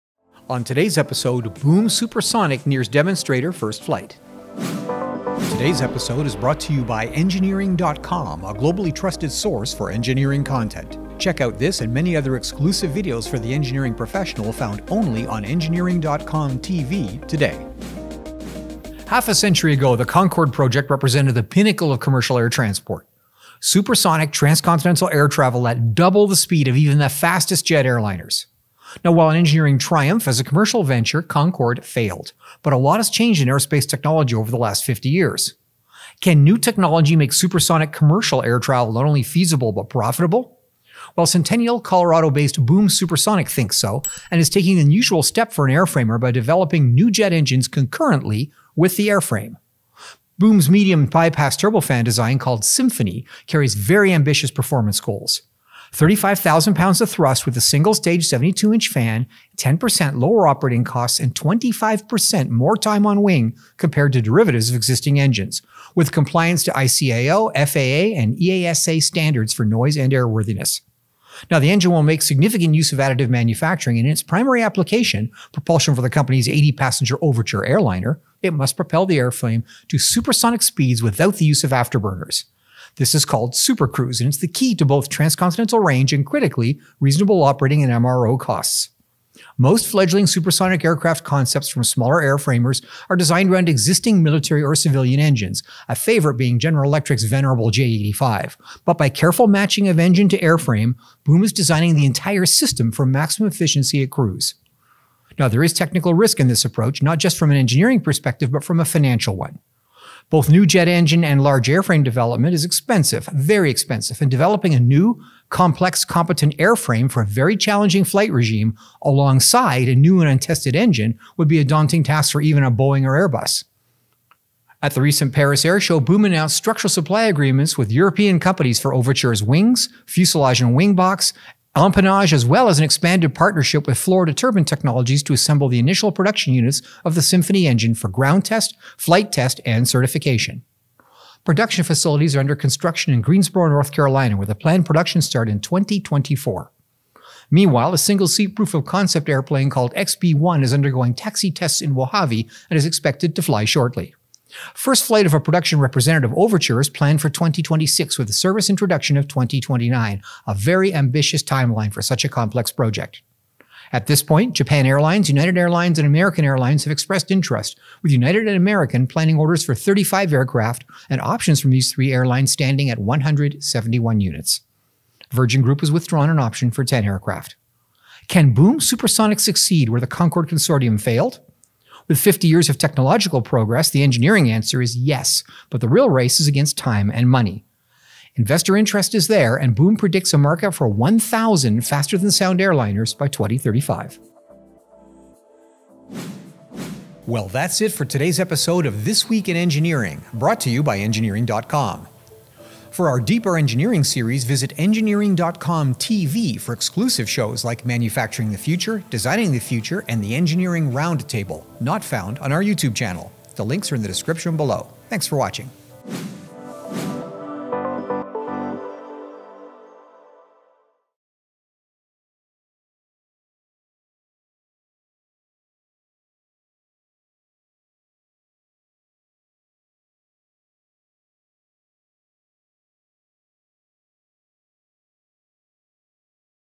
Boom Supersonic Nears Demonstrator First Flight
Regularly scheduled, commercial supersonic flight became a reality half a century ago with Concorde, but the high costs associated with supersonic aircraft technology doomed the project to commercial failure, despite its engineering excellence. Boom Supersonic is moving forward with an 80-seat composite faster-than-sound airliner using proprietary engines. Developing a novel airframe and engine both in-house is ambitious for any startup, but Boom h that is cheap that you help me of the sentence as commitments from three major airlines and has begun construction of a production facility in North Carolina.

STEM GAMES:
Play some fun engineering-based STEM games on engineering.com: